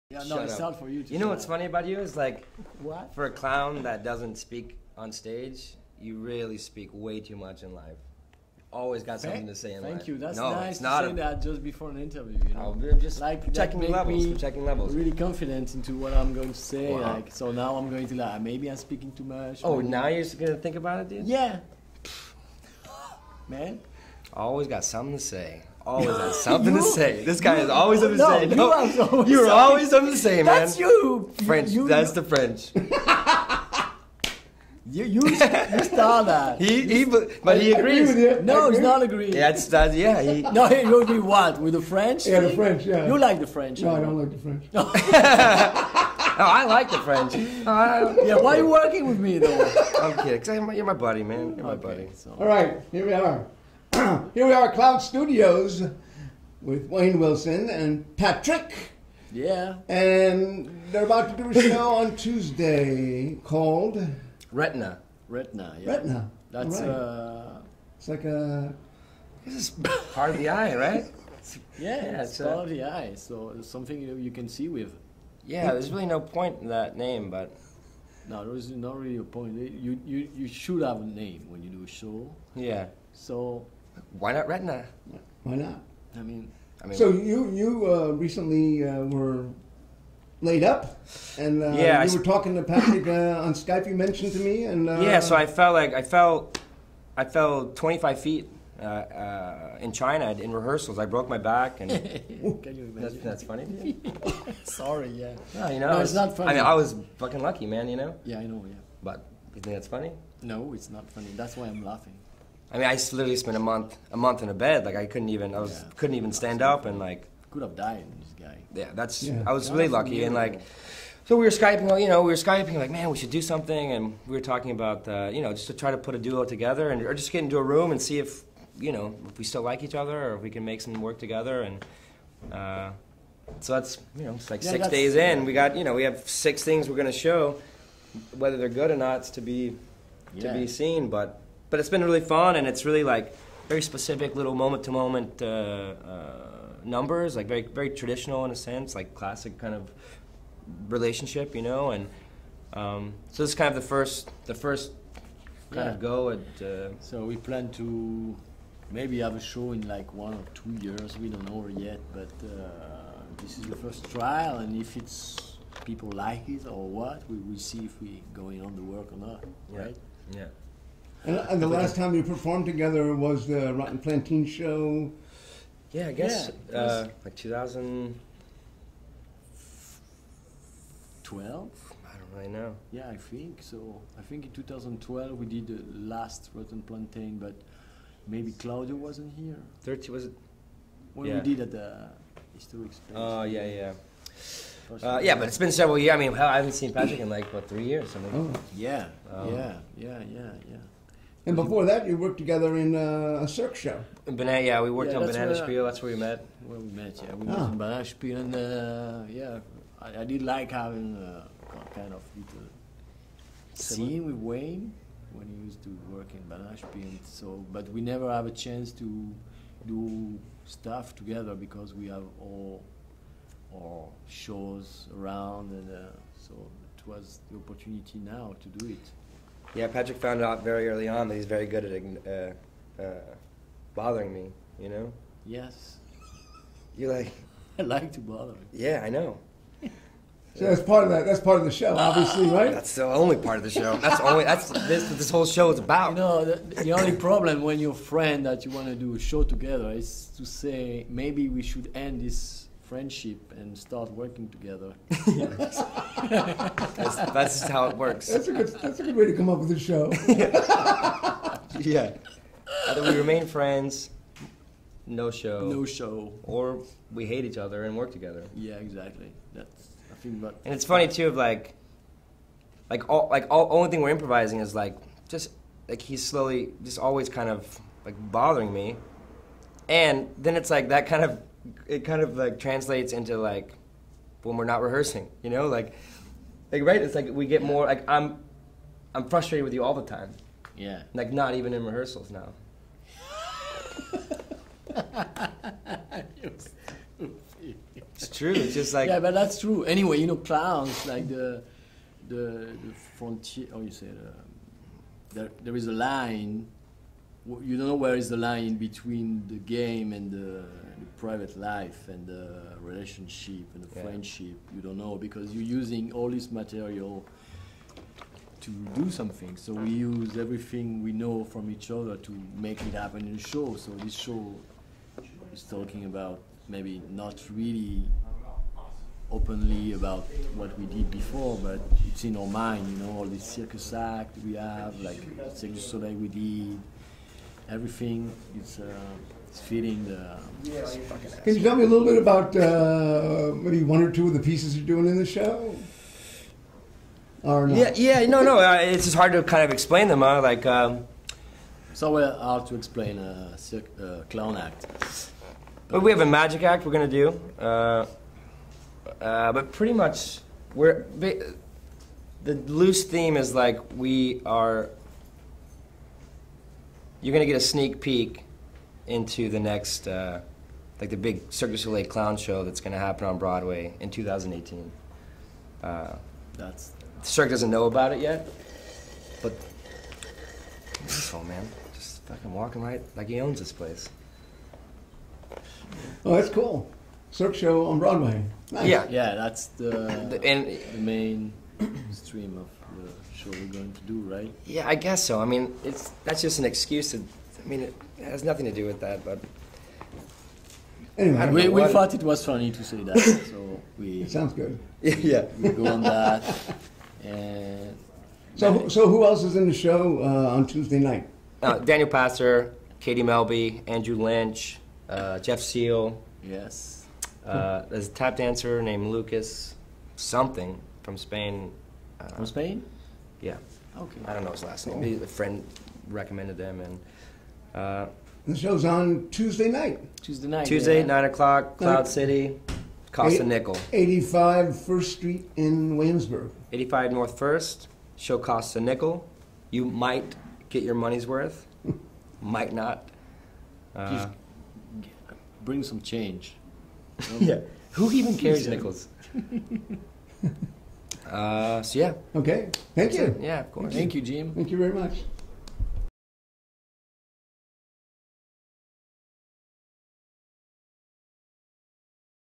Vaudevisuals Interview with Wayne Wilson and Patrick De Valette - "RETINA"
Vaudevisuals interview with clown/physical comedians Wayne Wilson and Patrick De Valette. Talking about their new show "Retina" which is having a showing at Cloud City June 23 at 9pm.
The show includes other performers such as Daniel Passer, Jeff Seal, Katie Melby and Andrew Lynch.
Cloud City
85 N. 1st Street
Williamsburg, Brooklyn 11249